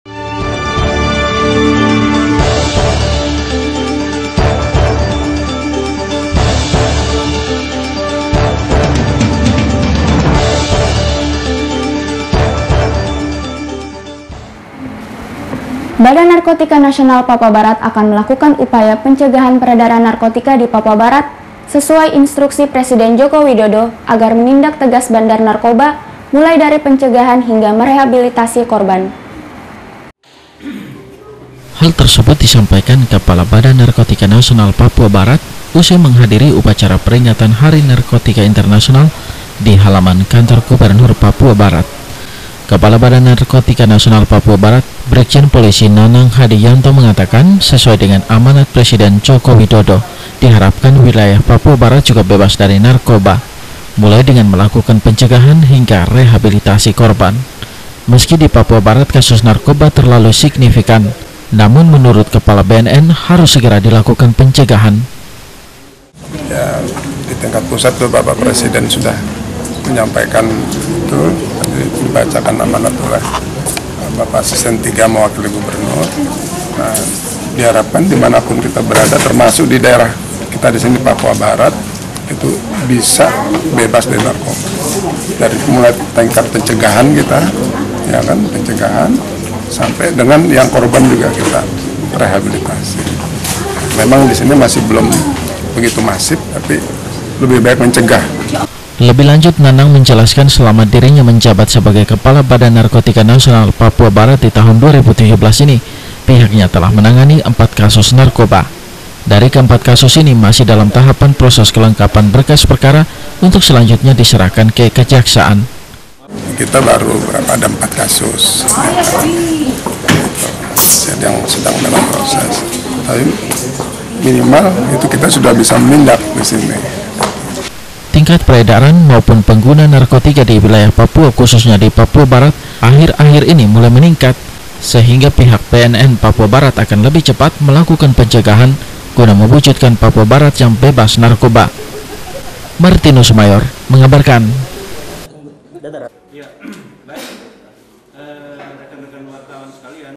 05 - KASUARI NEWS - WUJUDKAN PAPUA BARAT BEBAS NARKOBA BNN CEGAH PEREDARAN
Badan Narkotika Nasional Papua Barat Akan Melakukan Upayapencegahan  Peredaran Narkotika Di Papua Barat Sesuai Instruksi Presiden Joko Widodo Agara Menindak Tegas Bandar- Bandar Narkoba, Mulai Dari Pencegahan Hingga Merehabilitasi Korban.